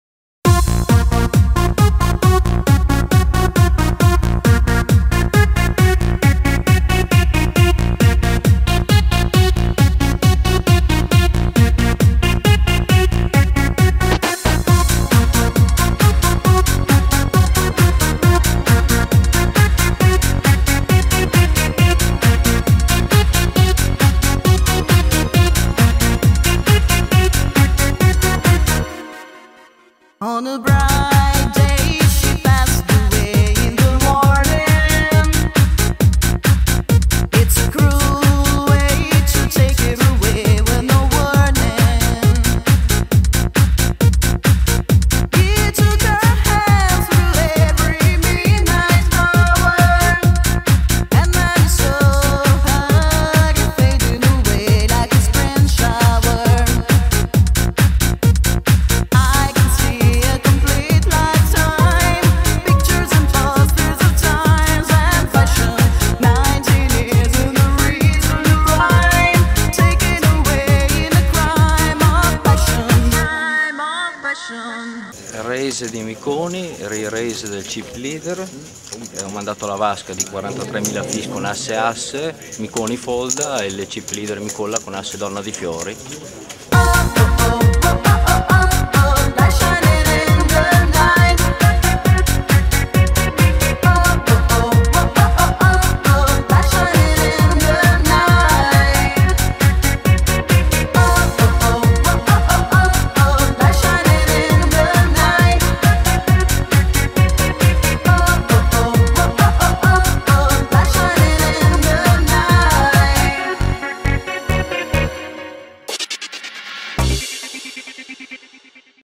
EPT Snowfest 2010 - Main event - Day 2 - Si vince e si perde!
Mani vinte mani perse e ... Luca Pagano out